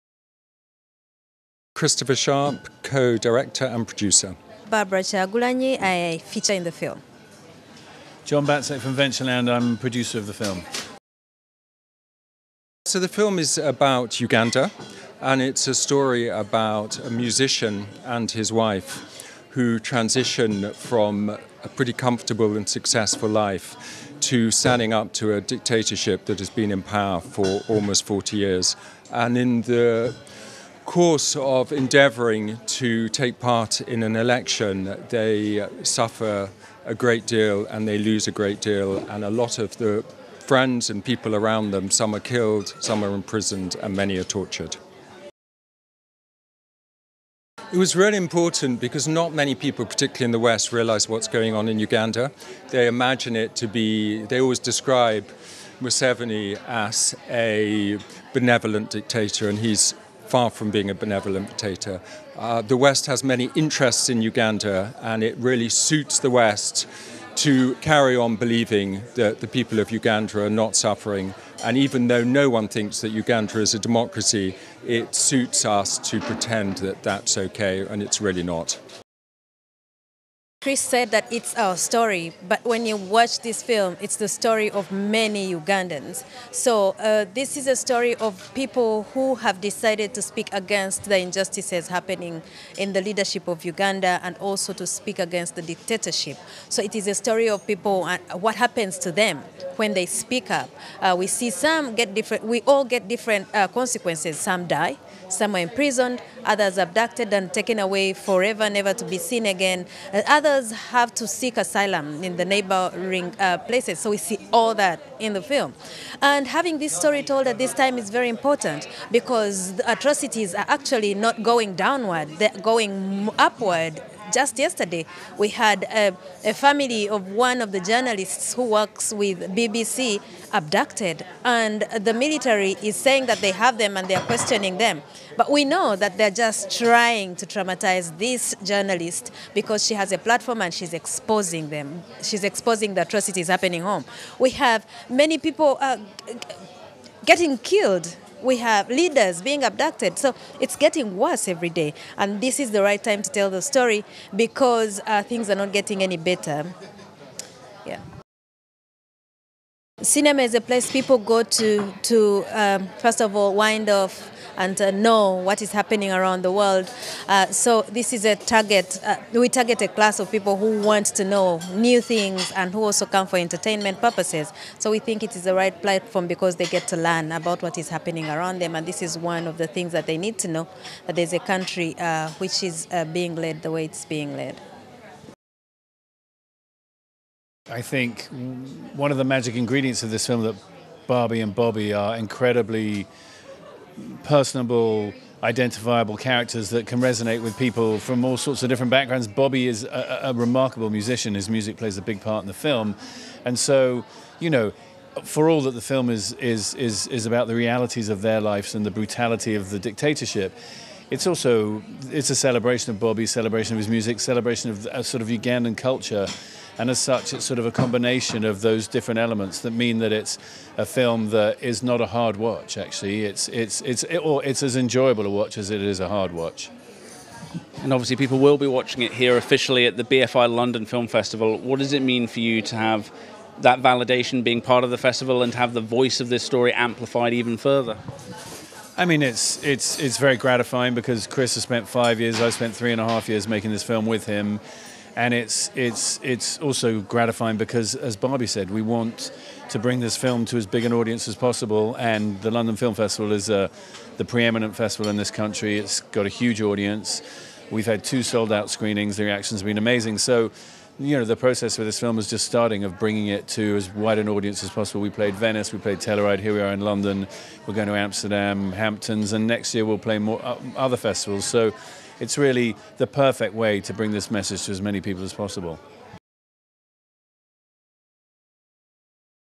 Bobi Wine: The People’s President Interview BFI London Film Festival 2022
Red Carpet News TV provides extensive interviews with directors and filmmakers at the 66th BFI London Film Festival 2022.